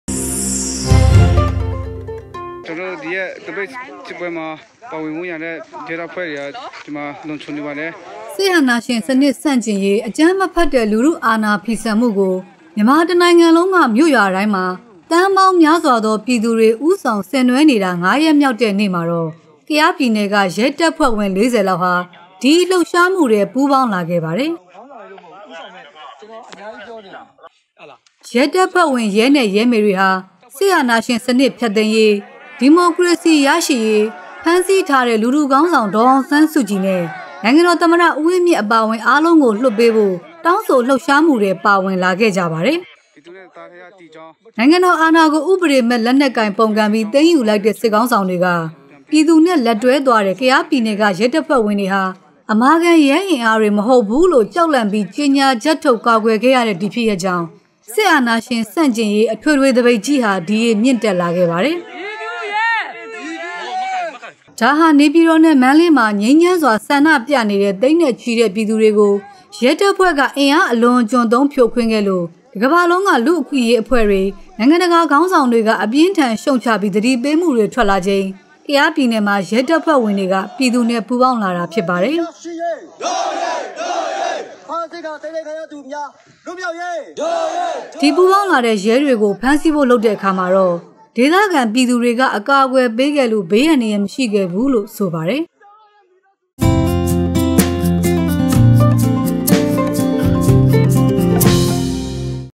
“ကယားပြည်နယ်မှာ ရဲတပ်ဖွဲ့ဝင် ၄၀ ပြည်သူ့နဲ့လက်တွဲပြီး စစ်အာဏာရှင်ကိုဆန့်ကျင်”
နိုင်ငံတော်အာဏာကို ဥပဒေမဲ့လက်နက်ကိုင်ပုန်ကန်ပြီး သိမ်းယူလိုက်တဲ့ စစ်ခေါင်းဆောင်တွေက ပြည်သူနဲ့လက်တွဲသွားတဲ့ ကယားပြည်နယ်က ရဲတပ်ဖွဲ့ဝင်တွေဟာ အမာခံရဲအင်အားတွေမဟုတ်ဘူးလို့ ကြောက်လန့်ပြီး ကြေညာချက်ထုတ် ကာကွယ်ခဲ့ရတဲ့ ဒီဖြစ်ရပ်ကြောင့် စစ်အာဏာရှင်ဆန့်ကျင်ရေး အထွေထွေသပိတ်ကြီးဟာ ဒီရေမြင့်တက်လာခဲ့ပါတယ်။